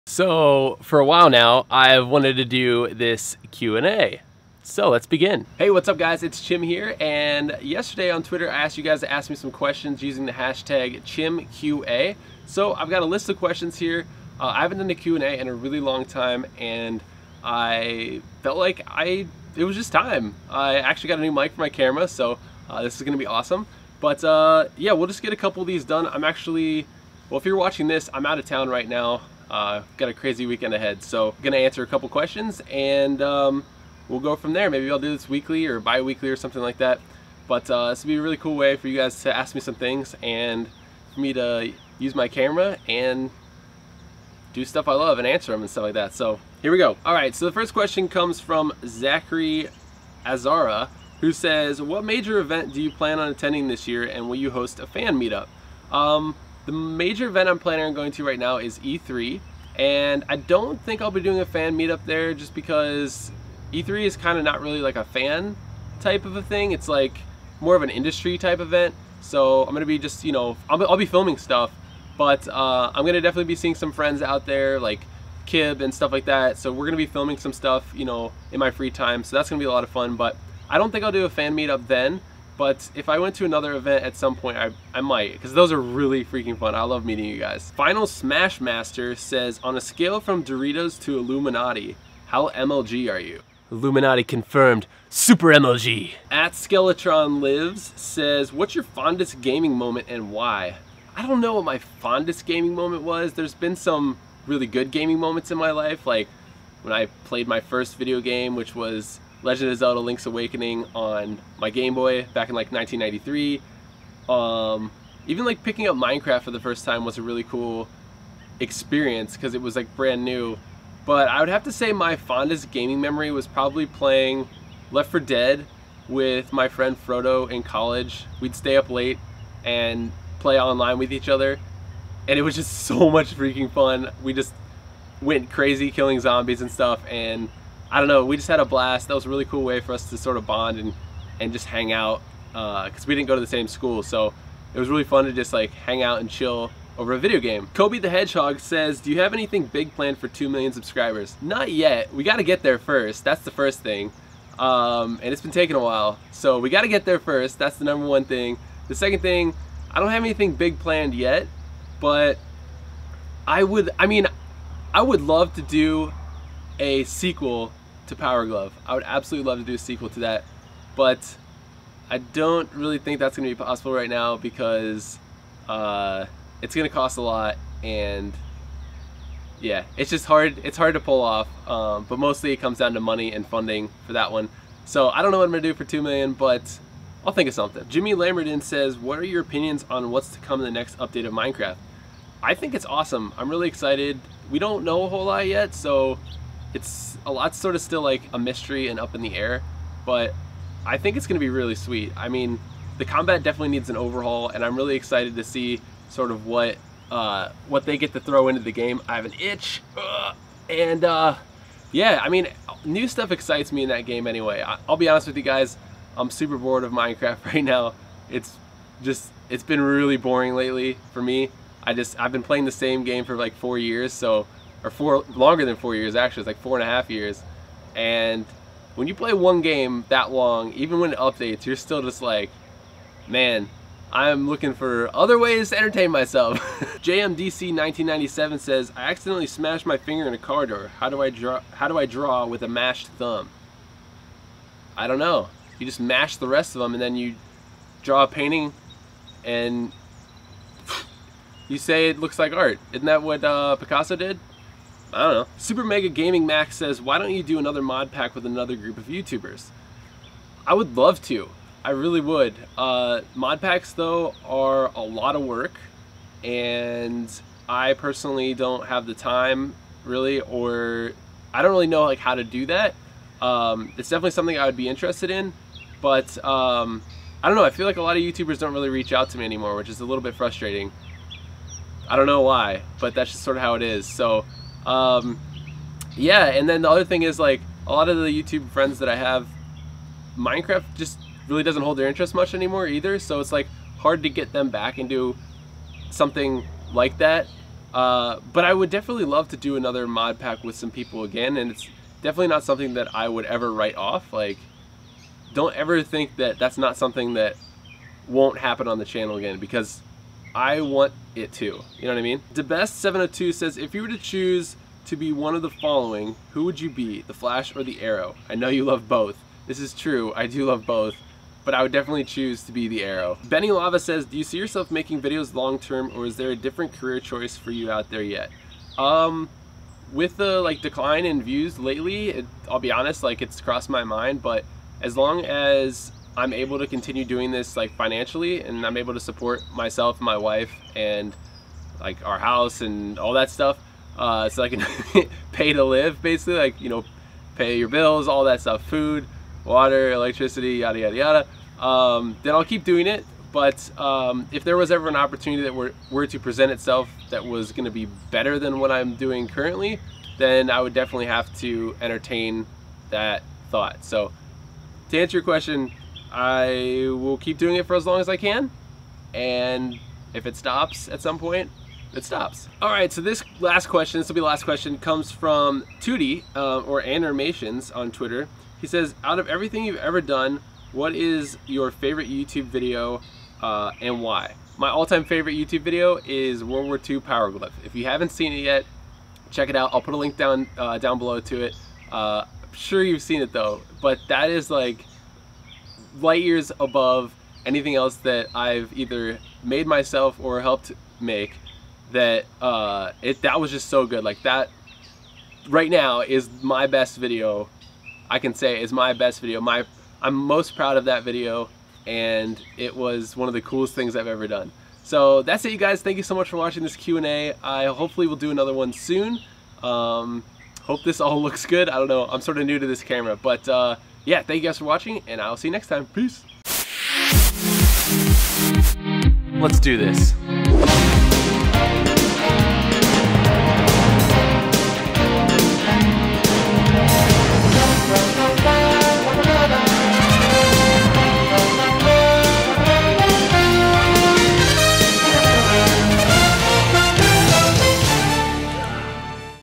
ANSWERING YOUR QUESTIONS!!! - Chim Q & A #1
Welcome to my backyard! Today I'll be answering some questions from you awesome Swifters from twitter (using the hashtag ChimQA). Hope you enjoy!